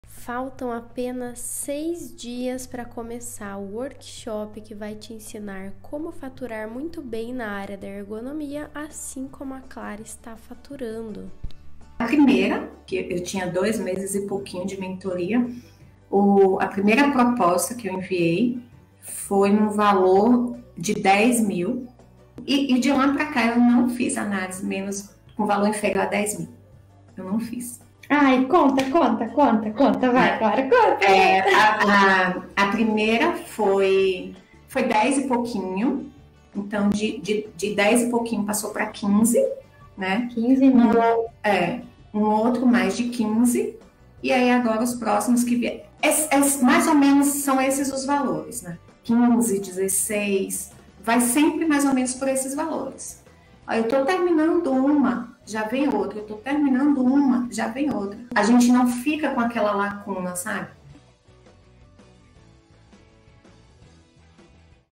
Faltam 6 dias para a Semana do Consultor de Ergonomia [CONTAGEM REGRESSIVA COM DEBORA DENGO]
⏳ Faltam 6 dias para a Semana do Consultor de Ergonomia

🗓️ CONTAGEM REGRESSIVA COM DEBORA DENGO

No workshop você vai aprender como faturar acima de R$10 mil por mês na área da ergonomia, mesmo começando do zero!

🟢 Participe do grupo de whats app do evento para não perder nada